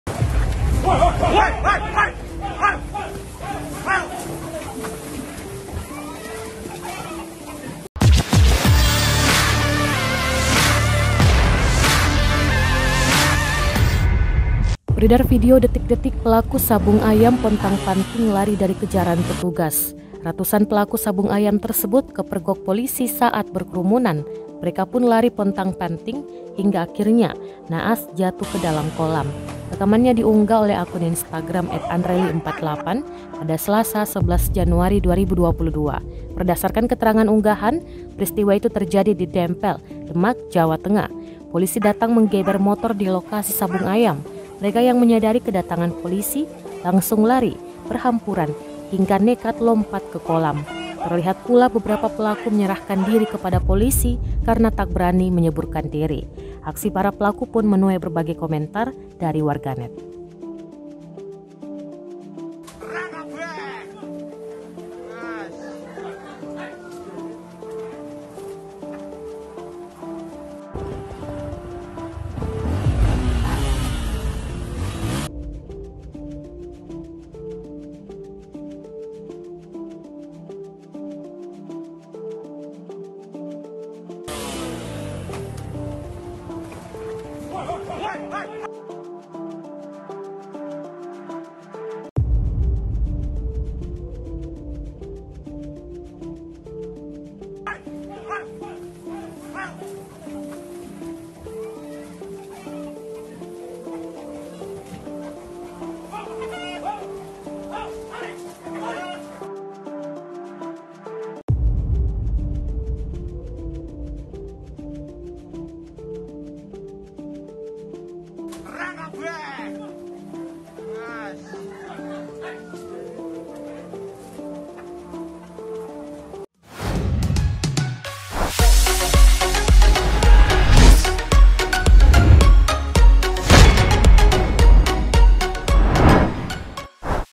Ratusan Pelaku Sabung Ayam Pontang Panting Lari Sampai Tecebur saat Digeber Polisi
TRIBUN-TIMUR.COM - Beredar video detik-detik pelaku sabung ayam pontang-panting lari dari kejaran petugas.

Ratusan pelaku sabung ayam tersebut kepergok polisi saat berkerumunan.

Mereka pun lari pontang-panting hingga akhirnya naas jatuh di dalam sawah.

Rekamannya diunggah akun Instagram @andreli_48 pada Selasa, 11 Januari 2022.

Berdasarkan keterangan unggahan peristiwa itu terjadi di Dempel, Demak Jawa Tengah.

Polisi datang menggeber motor di lokasi sabung ayam.

Mereka yang menyadari kedatangan polisi langsung lari berhamburan hingga nekat lompat ke sawah.

Terlihat pula beberapa pelaku menyerahkan diri kepada polisi karena tak berani menyebutkan dirinya

Aksi para pelaku pun menuai berbagai komentar dari warganet.

Narator Video: Nur Fajriani

(TRIBUN-TIMUR.COM)